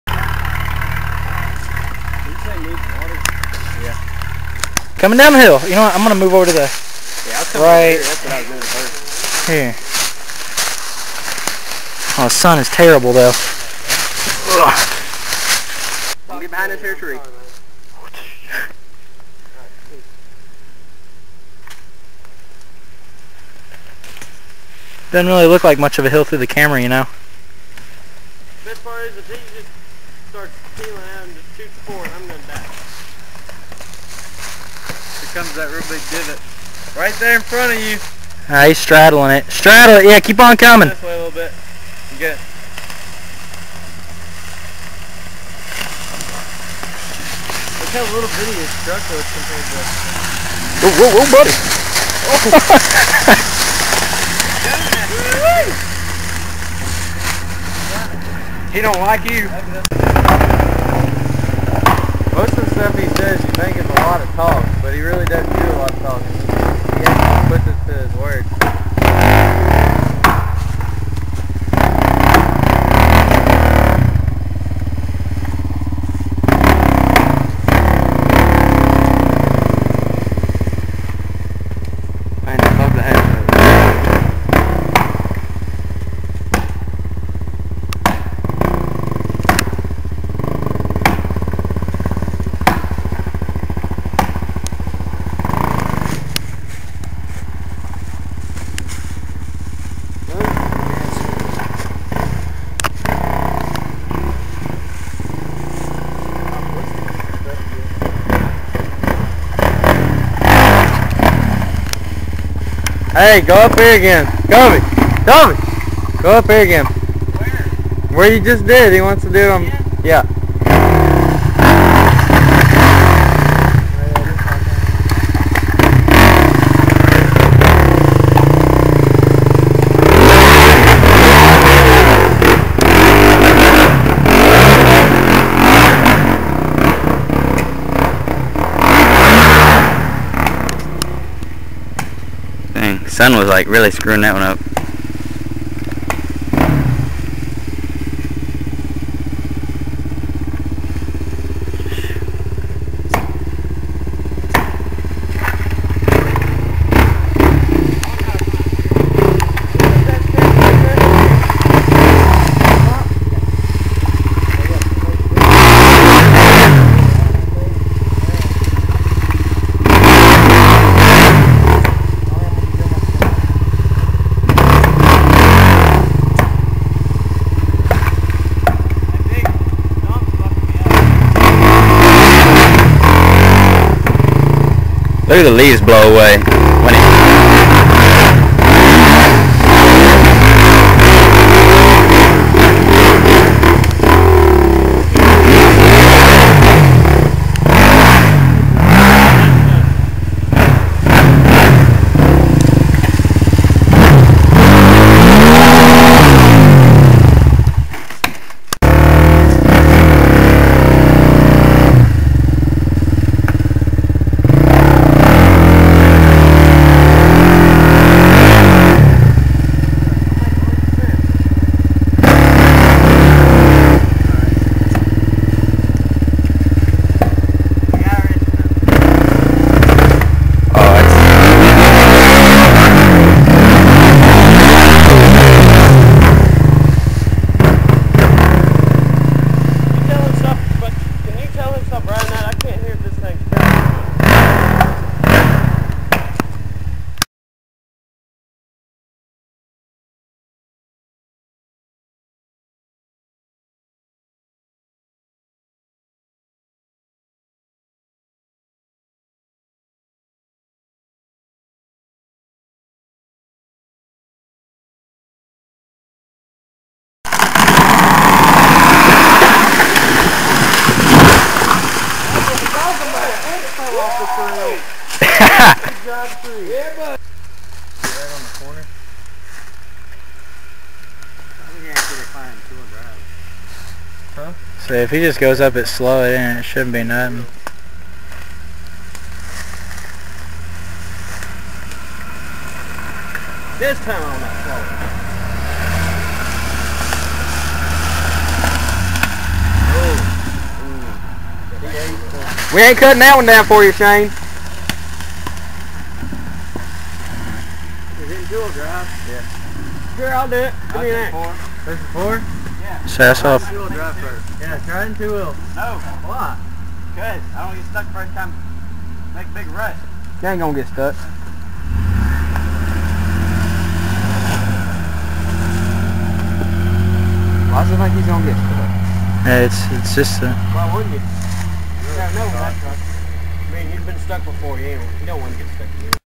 A Day At Kyle's Farm
1995 GMC Sonoma
Kawasaki KFX
a little bit of trail blazing